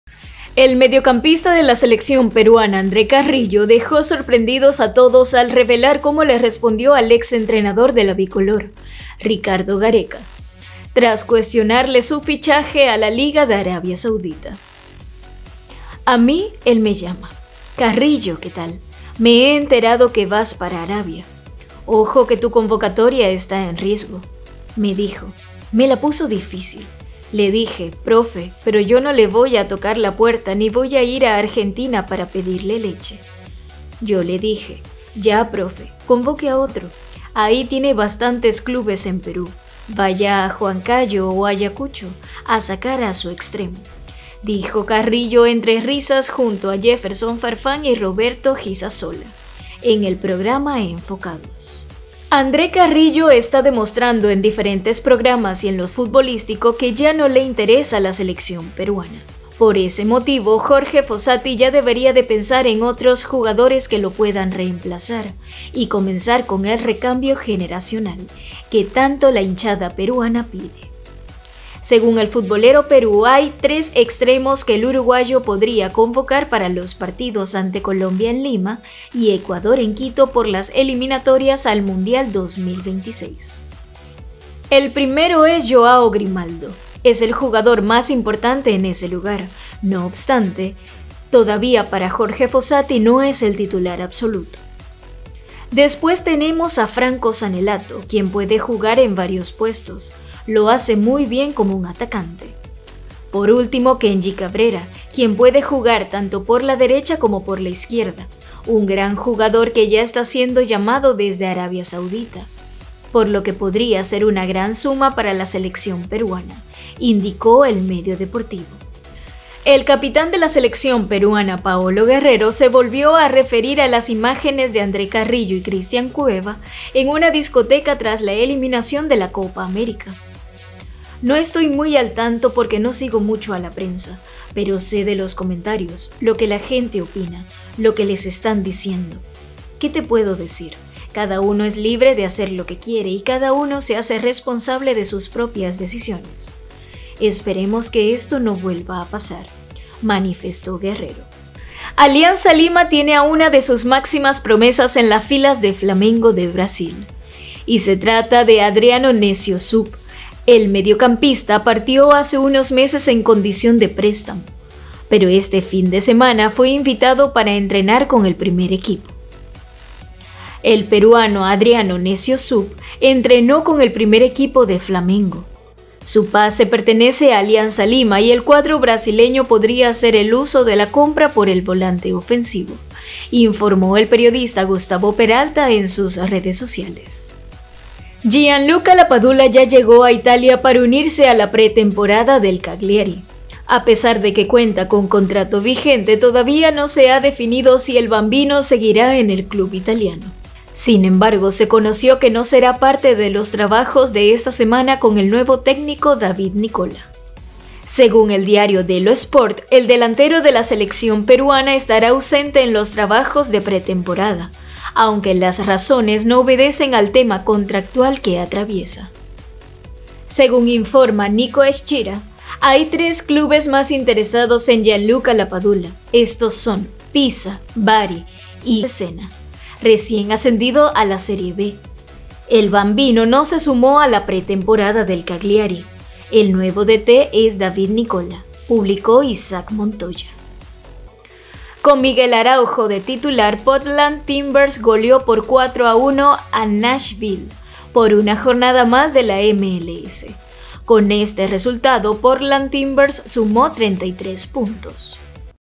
3 EXTREMOS QUE CONVOCARÁ JORGE FOSSATI A LA SELECCION PERUANA, ANDRE CARRILLO A RICARDO GARECA
3 EXTREMOS QUE CONVOCARÁ JORGE FOSSATI A LA SELECCION PERUANA, ANDRE CARRILLO A RICARDO GARECA